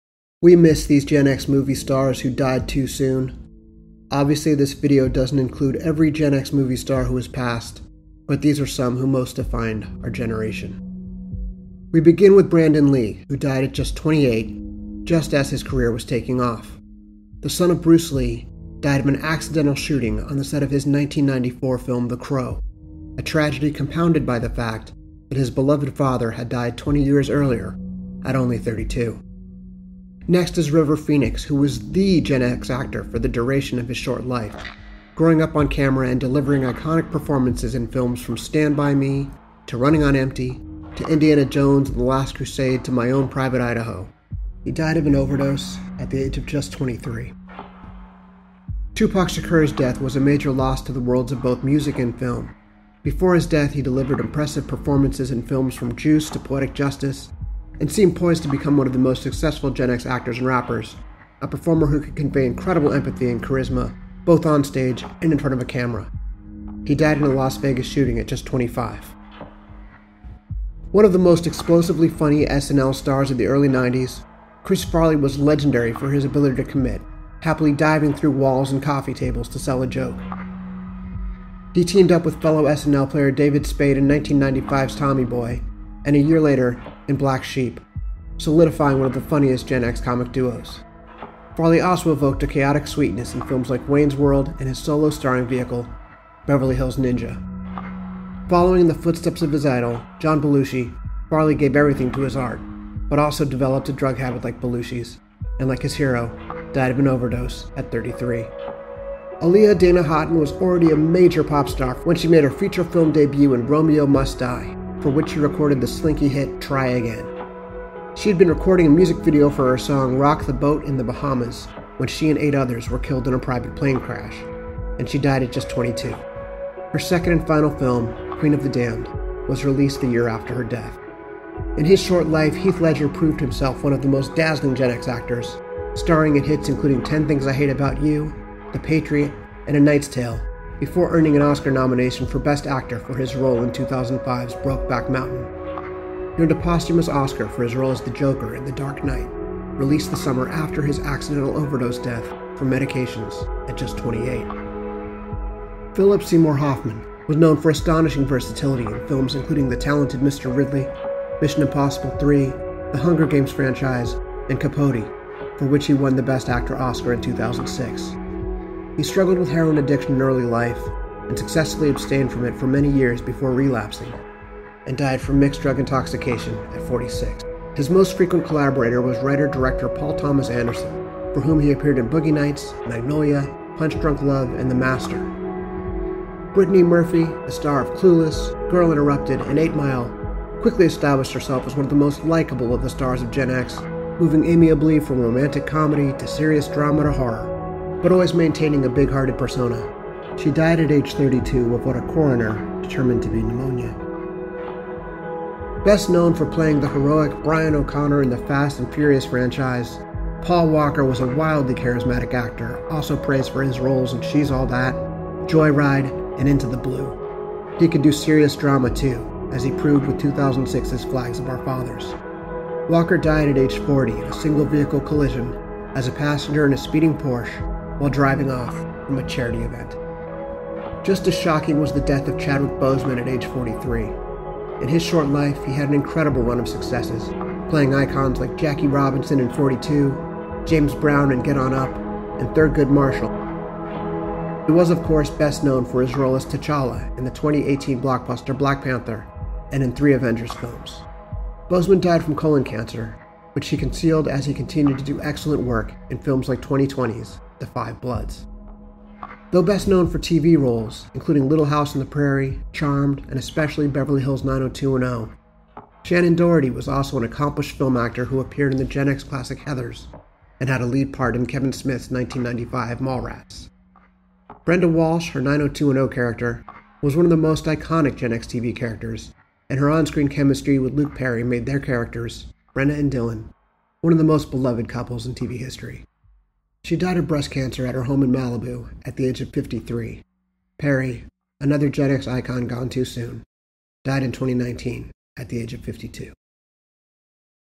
10 Gen X Movie Stars Gone Too Soon
The death of Shannen Doherty inspires us to look back at some of the other key figures of our generation we who lost too soon, to illness, drugs, violence, and other tragedies.

They include Brandon Lee, the first notable Gen X figure to die young, as a result of a tragic on-set accident during his spectacular leading debut in The Crow. Compounding the tragedy was that it came after the death of his beloved father, martial arts and cinematic icon Bruce Lee.

Some Gen X movie stars made it to middle age before dying too soon, including the great Philip Seymour Hoffman, who demonstrated his astonishing range in films from Boogie Nights to The Master, both of which were directed by his longtime collaborator, Paul Thomas Anderson.

This list doesn't include every Gen X movie star gone too soon — the list is sadly too long. But it does include many of those who most shaped our culture and came to signify our generation in their too-short time on this earth.

This is, sadly, far from a complete list.